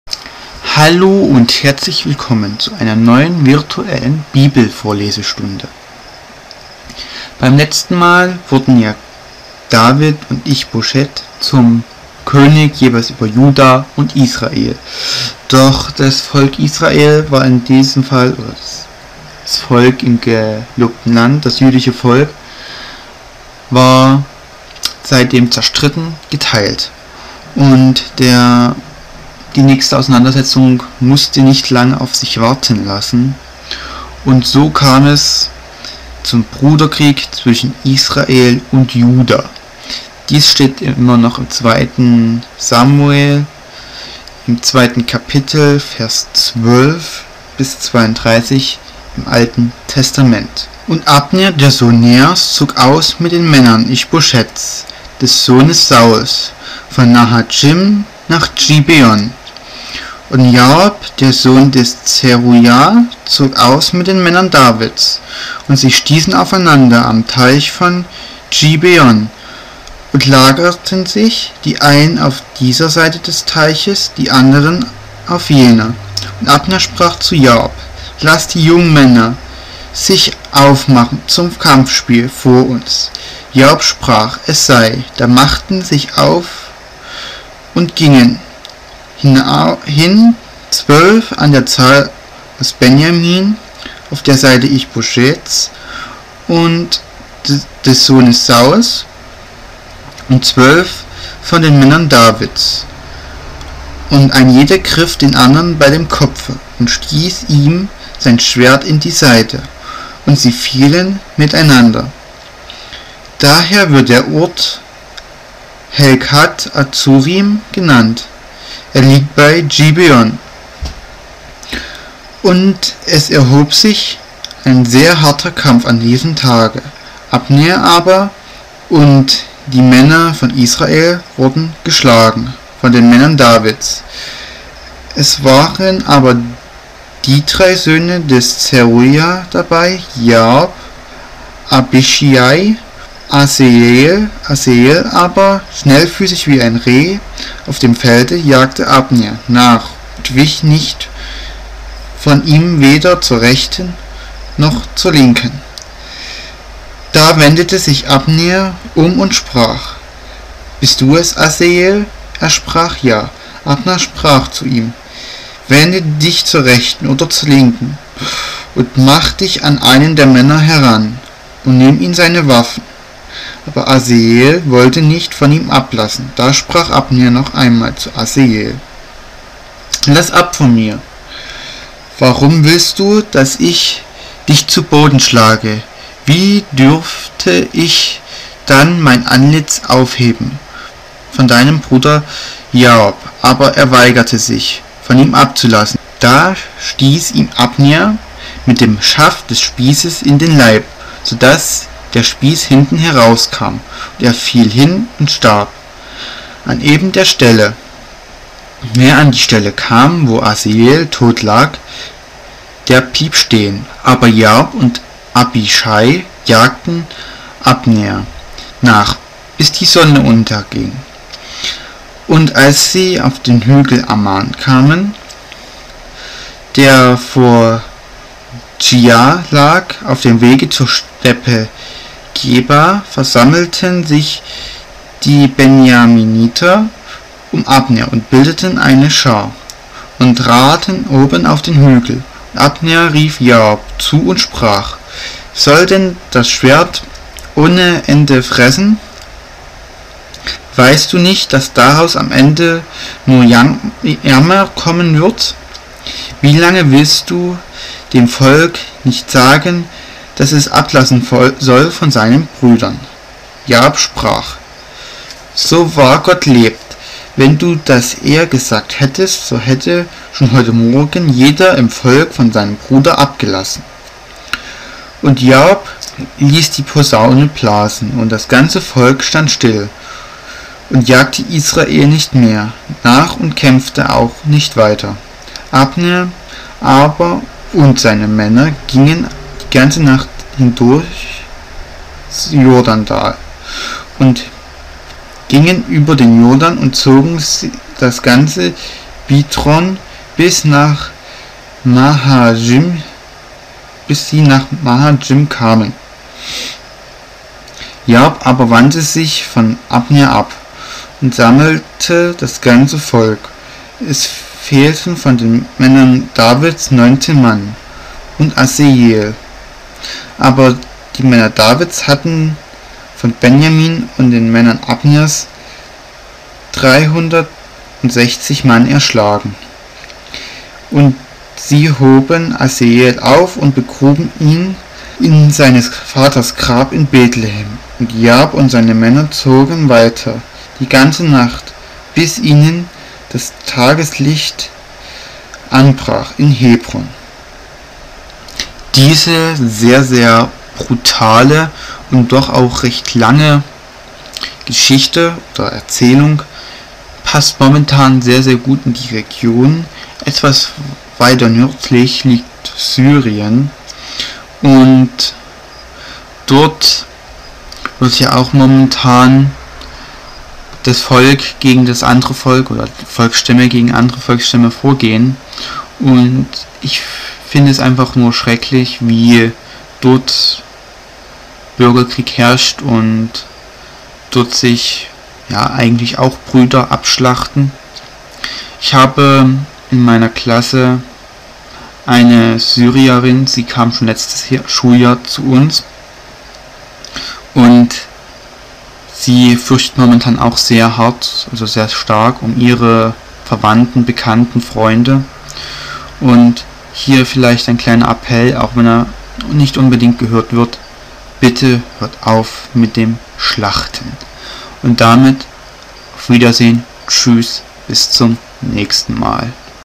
Bruderkrieg zwischen Israel und Juda | 2. Sam 2, 12-32 [Deutsch] [HD]
Ein sinnloser und schrecklicher Krieg.
Aus dem Alten Testament 2. Samuel 2, 2-32

Gelesen aus:
Lutherbibel für dich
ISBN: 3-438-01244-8
Deutsche Bibelgesellschaft

Kommentiert, gelesen und produziert von Raphael Robertoson 2012